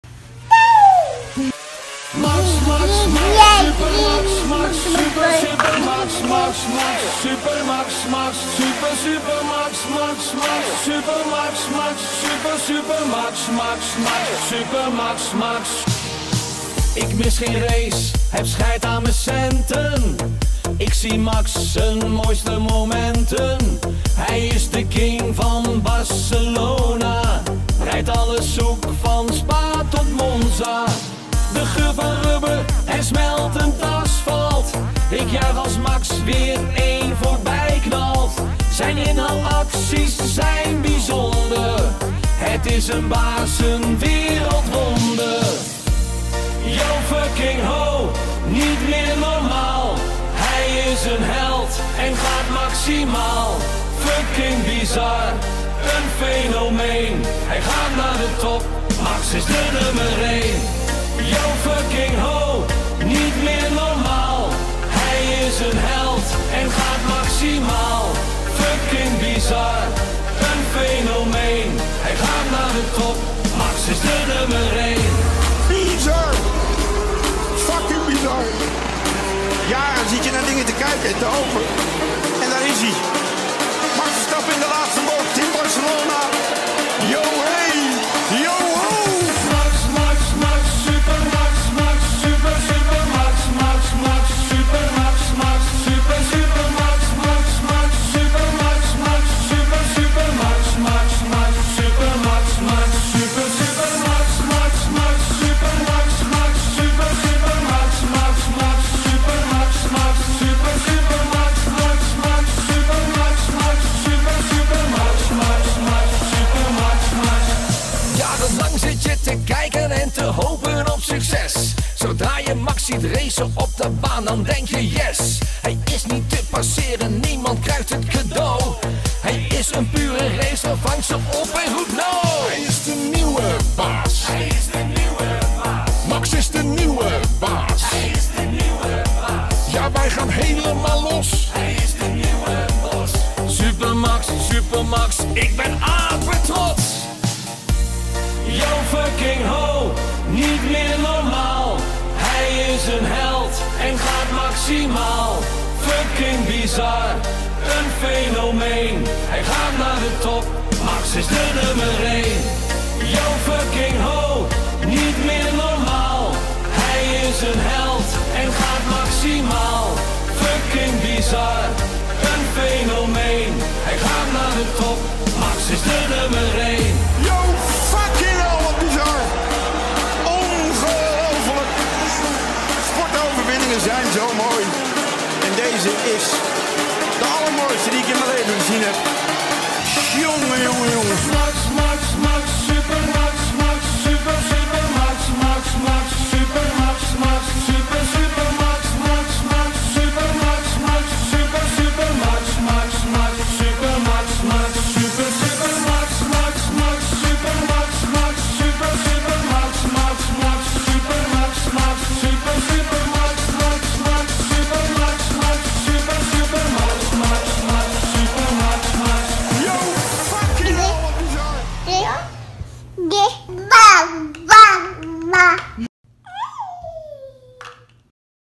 Super MAX
Videos of Max that me and my mom took of him during our trip to Indiana for his 1st Birthday 🥳❤️